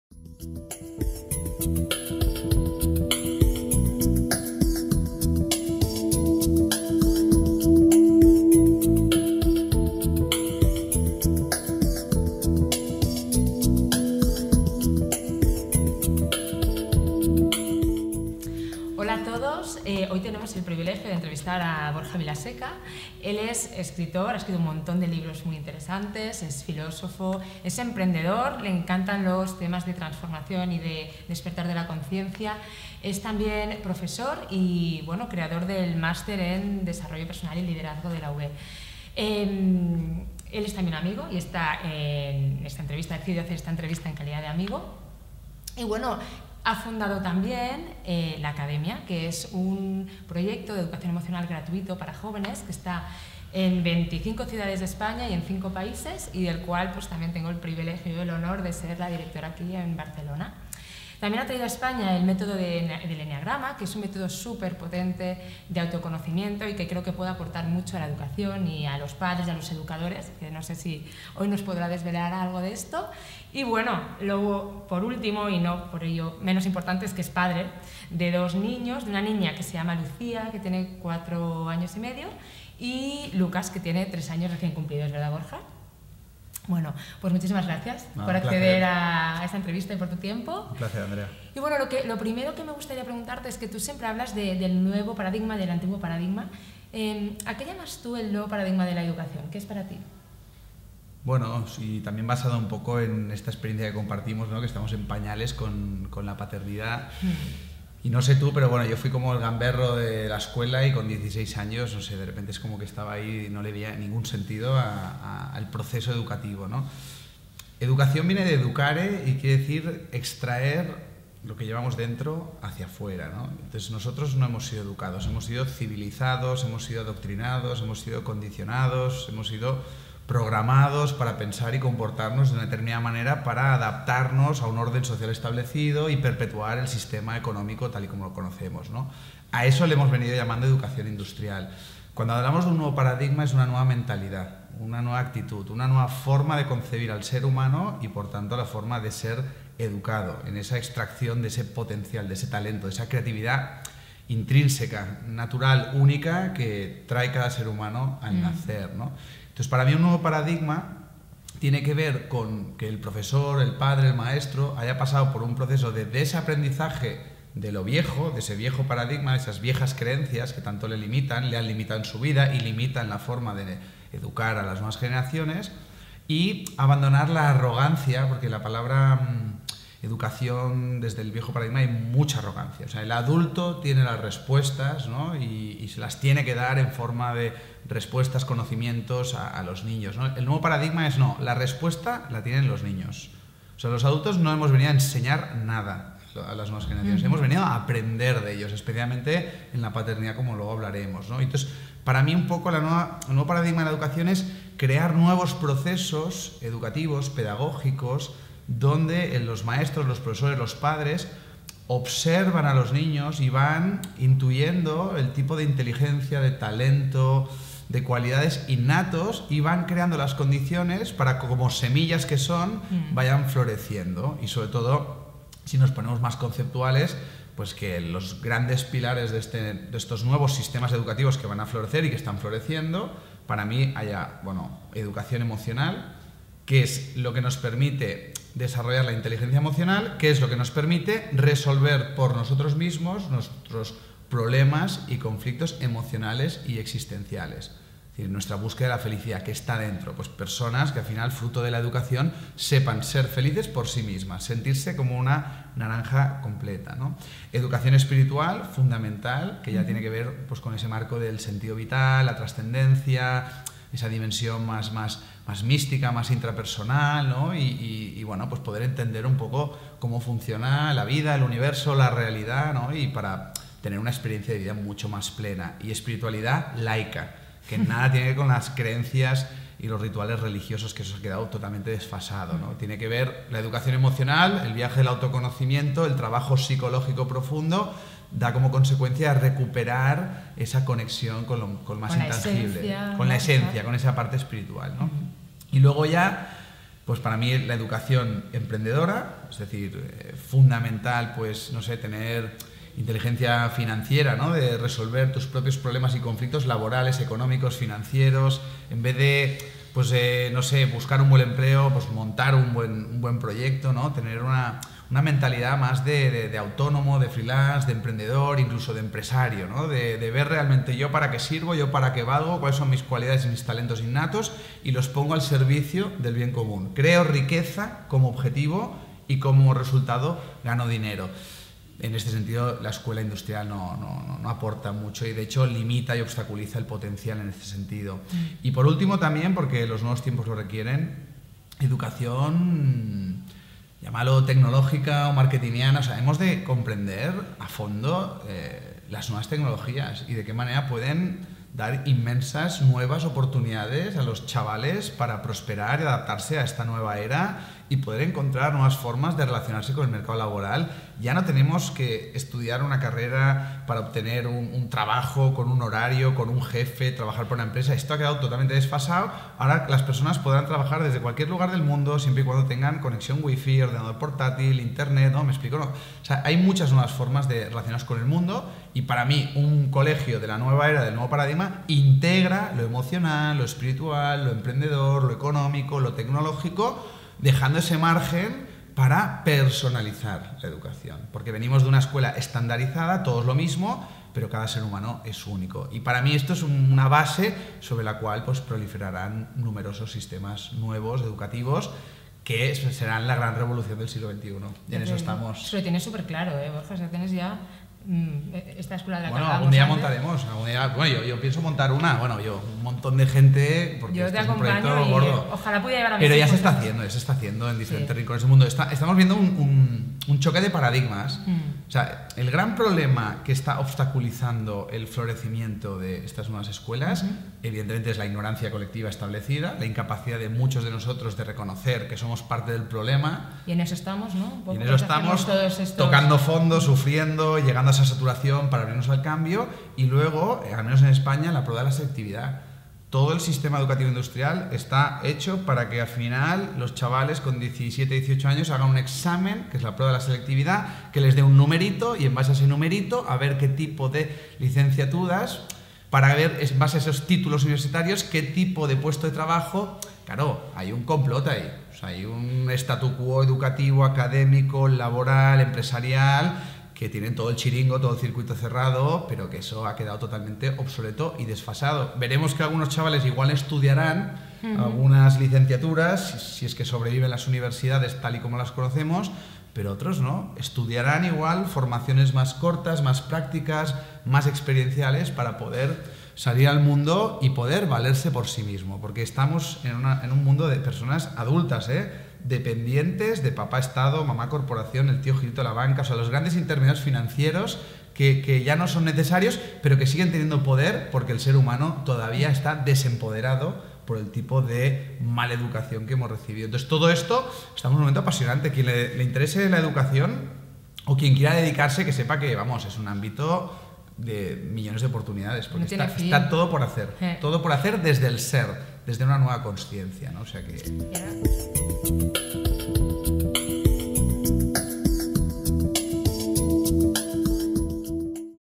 El nuevo paradigma educativo | Entrevista con Andrea Zambrano | Borja Vilaseca
Este vídeo es un fragmento de la entrevista que Andrea Zambrano, de Coaching AEIOU, le hizo a Borja Vilaseca, con quien reflexionó sobre el nuevo paradigma educativo. y